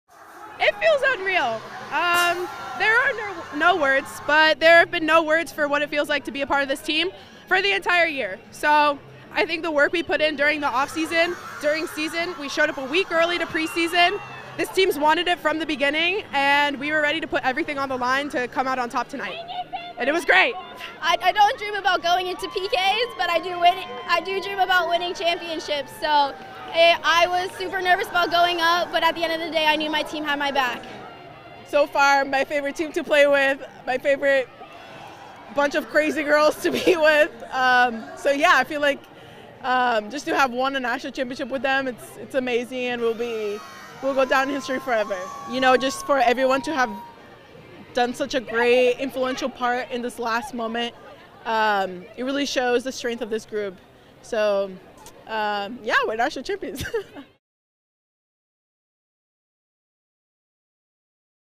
'We'll go down in history forever' : Stanford players discuss winning NCAA title
Stanford women's soccer players discuss their 2019 NCAA championship, which they won in a thrilling penalty kick shootout. Redshirt freshman Katie Meyer, junior Kiki Pickett, and junior Catarina Macario talk about what it means to win the NCAA title, the third in team history.